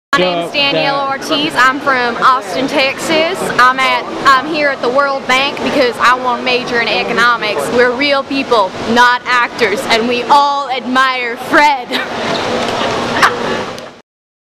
TESTIMONIAL - DANIELLE ORTIZ | UN YOUTH DELEGATE
I met Danielle at my time at the World Bank Group Seminar. Danielle is interested in issues of fraud and corruption associated with WBG projects, global economics and the impact of climate change on the global economy. She was also